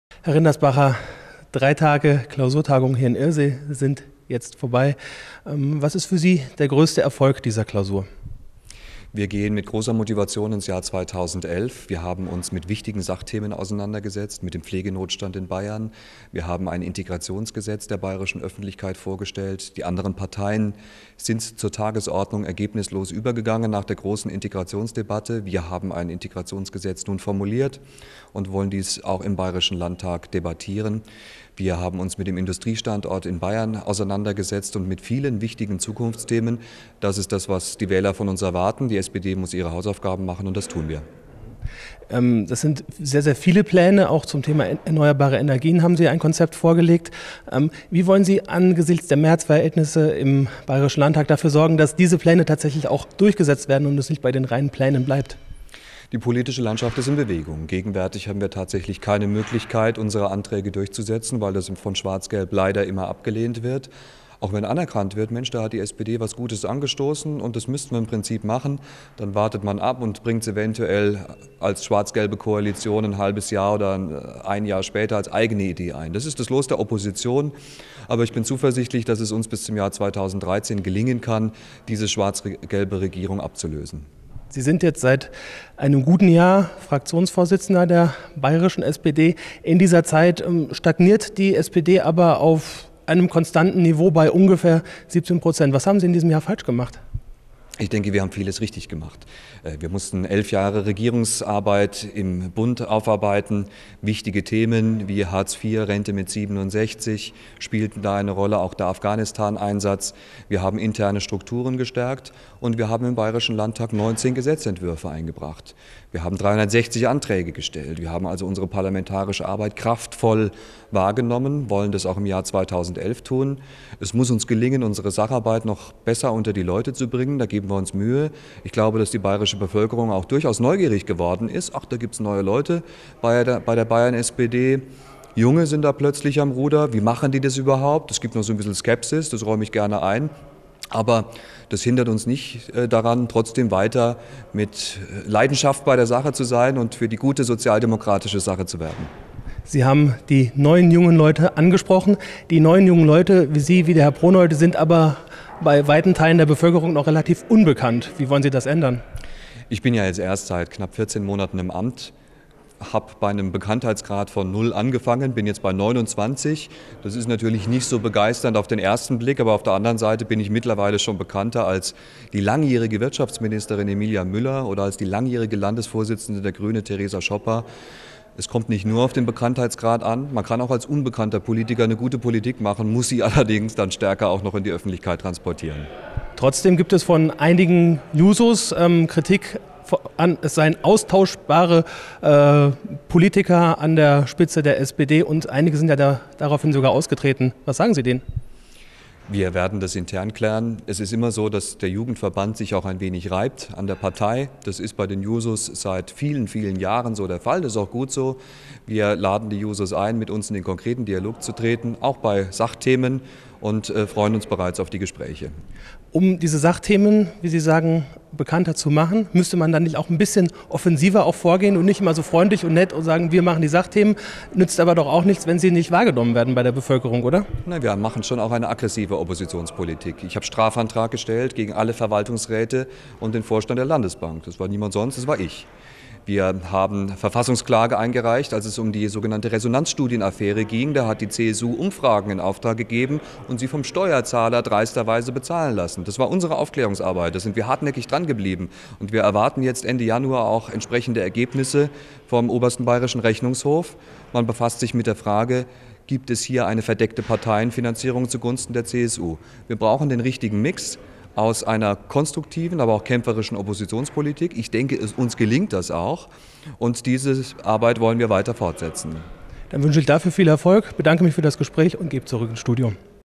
Im Gespräch mit Markus Rinderspacher
Wie die SPD in diesem Jahr ihre Sachthemen in diesem Jahr an den Wähler bringen will, darüber hat sich mein Kollege Sebastian Zausch im Anschluß an die SPD-Klausurtagung mit SPD Fraktionschef Markus Rinderspacher unterhalten.